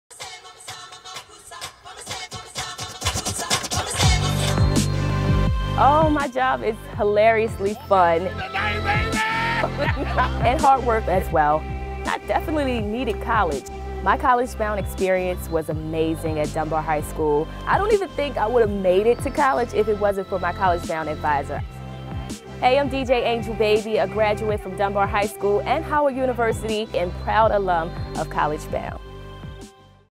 CollegeBound PSA_DJ Angel Baby
The incredible DJ Angel Baby is a graduate of Paul Laurence Dunbar High School, where she was part of the CollegeBound Foundation program. Afterwards, she graduated from Howard University and is now rising star in the world of radio and television.